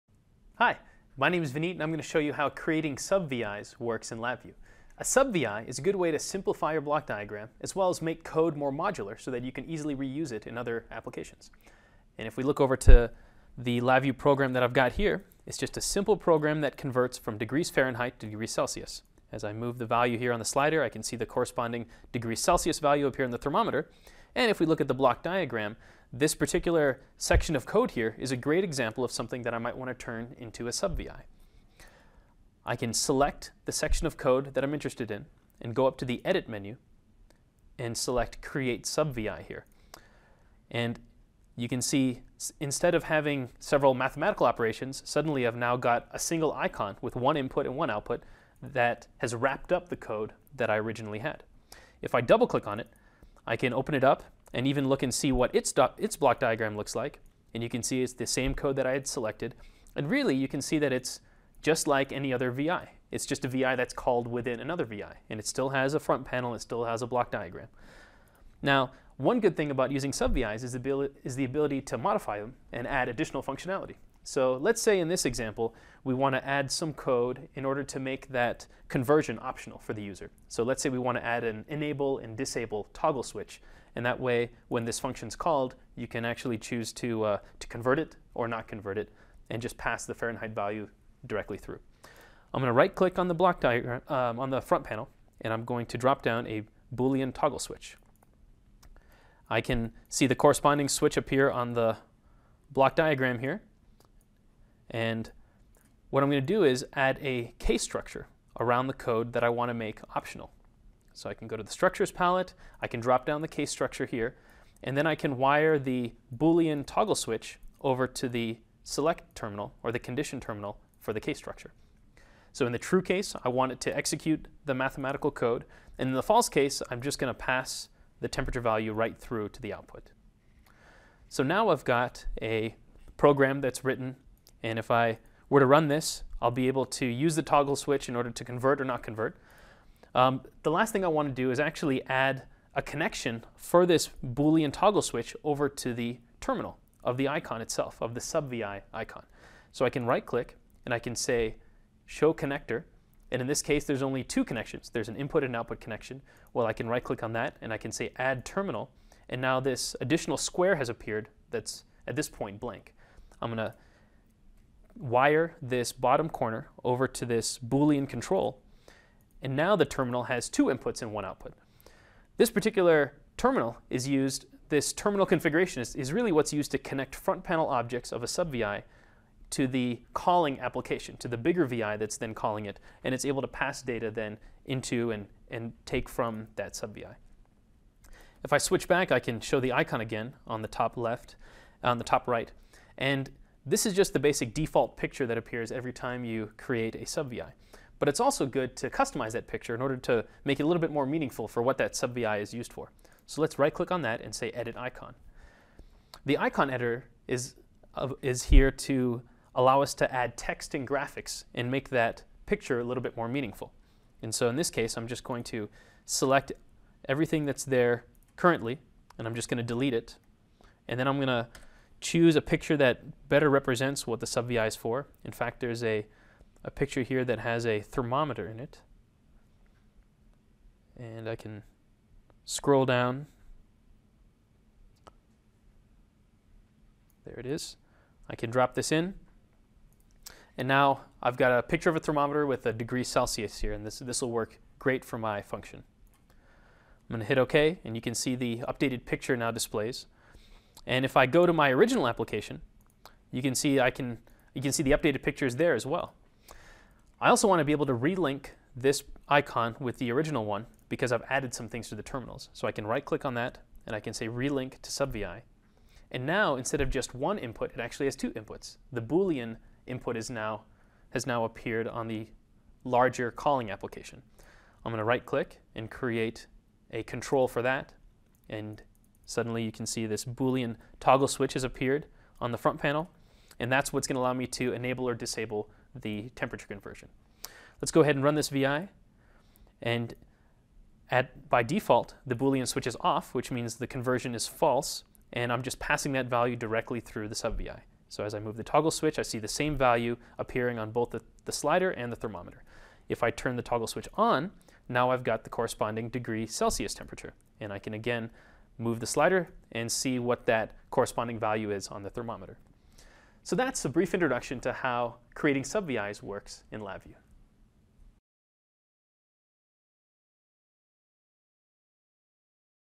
Creating Sub VIs in NI LabVIEW
Learn how to simplify block diagrams and make your code more modular by creating sub Vis in LabVIEW.